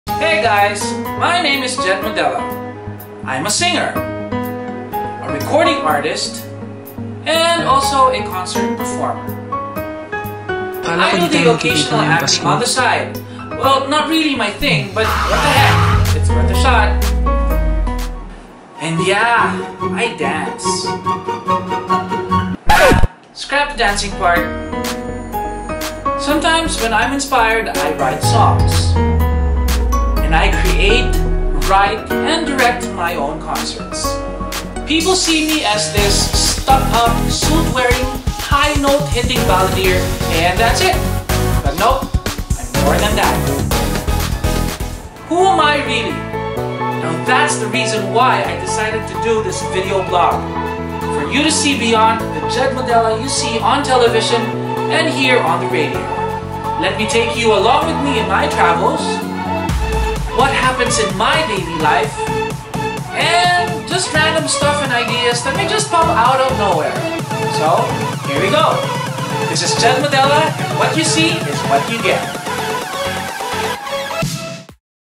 Who Am I Really?
Finally, the beginning of something new for me! People have been stuck to the idea of me as the singer who hits high notes. But seriously, I'm more to that, you know. Who am I really? These vlogs will show you everything... a first hand look on how it is to be in my shoes.